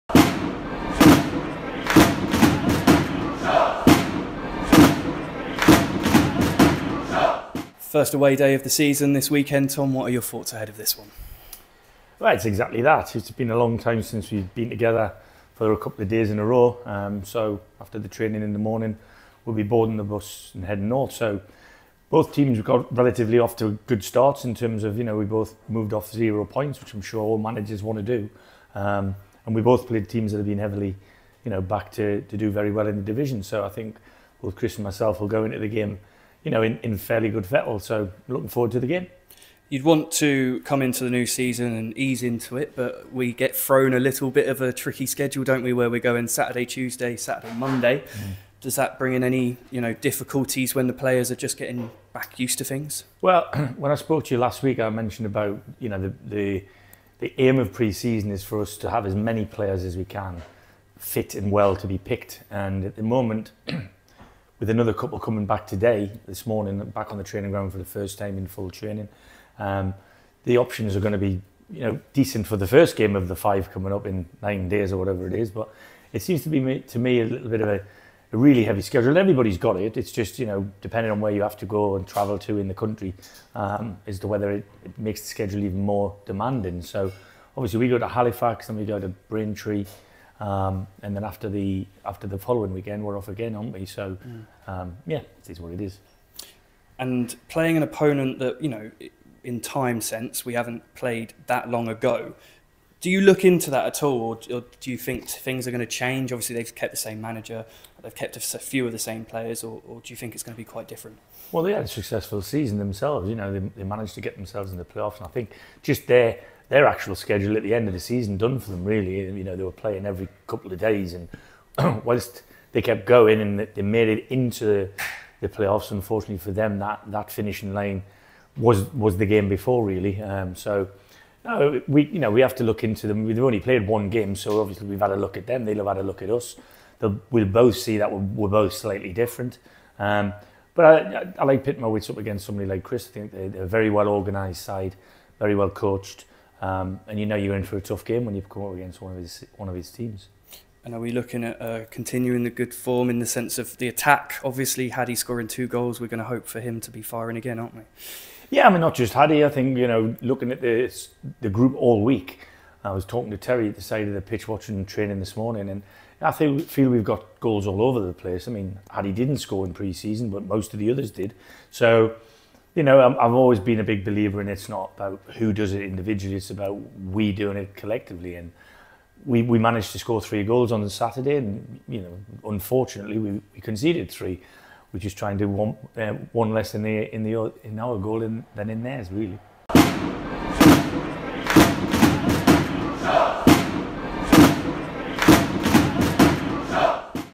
TOMMY WIDDRINGTON PRE-MATCH: FC Halifax Town (A)
Tommy Widdrington speaks to ShotsTV ahead of our trip to FC Halifax Town this weekend!